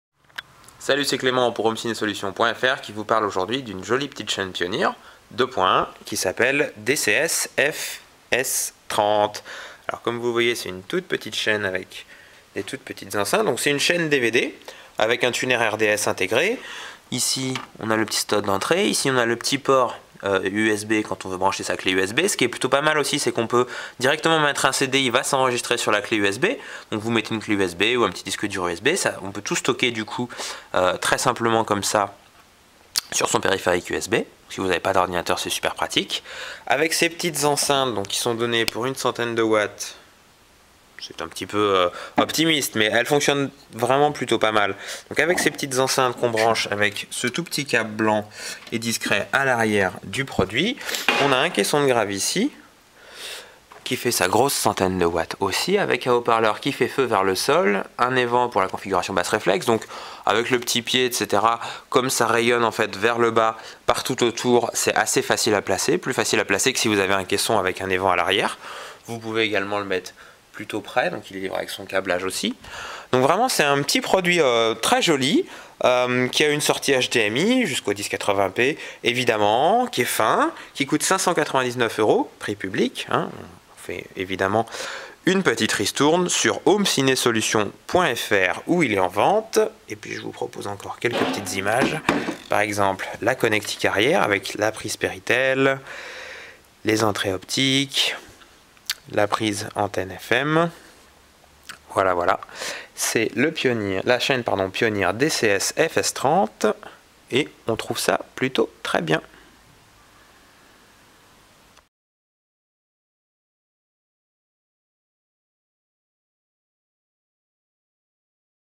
Pioneer DCS-F530
Découvrez une petite chaîne DVD home-cinéma 2.1 compacte et élégante, avec sortie HDMI et port USB, signée Pioneer.

évidemment !